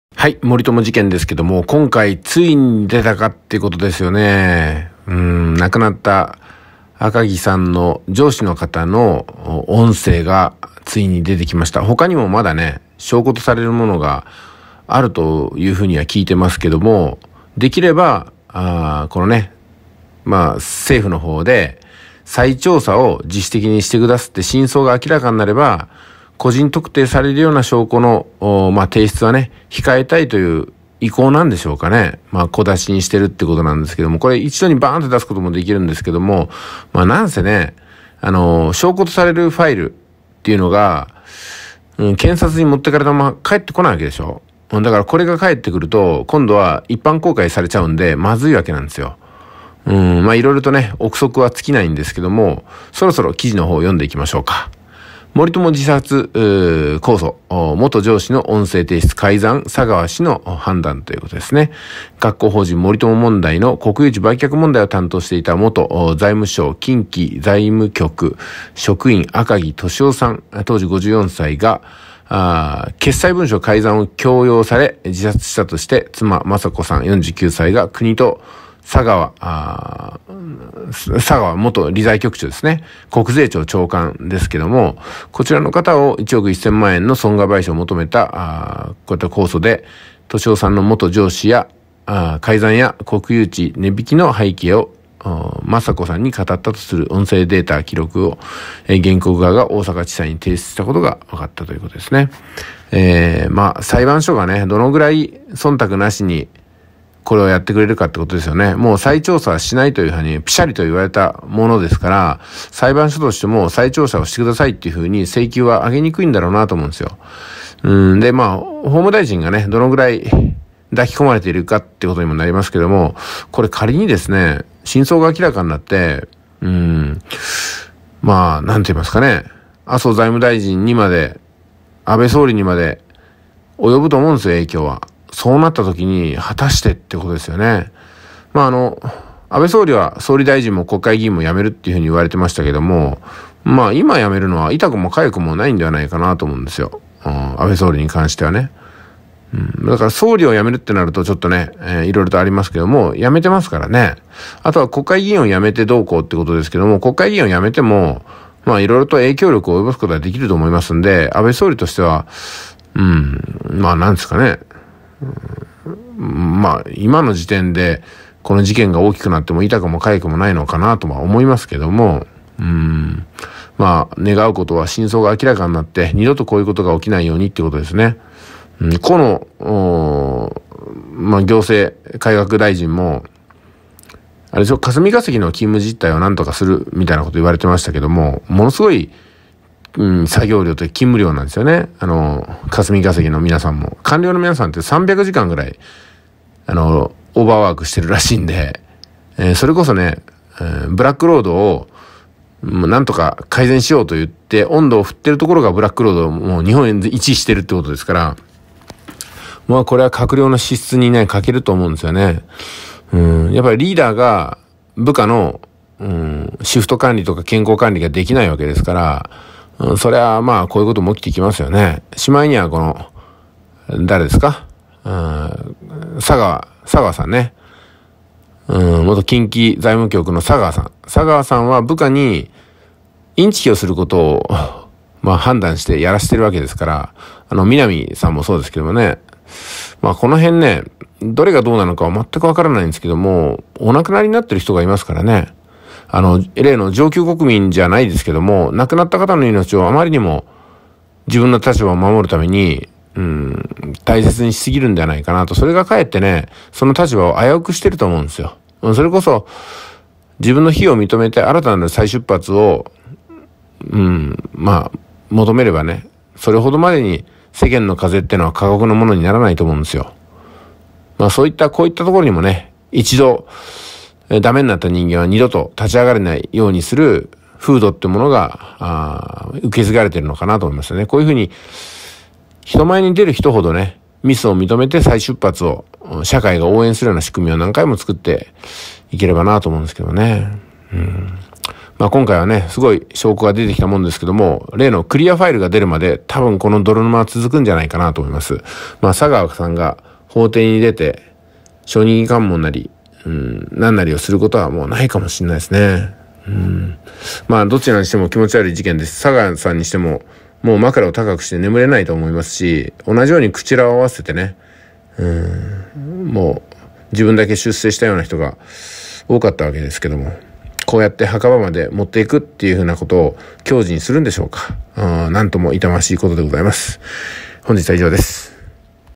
【隠居TV】森友自殺訴訟、元上司の音声提出 改ざん「佐川氏の判断」
森友自殺訴訟、元上司の音声提出　改ざん「佐川氏の判断」

「件名：速稼塾（あなたのお名前）」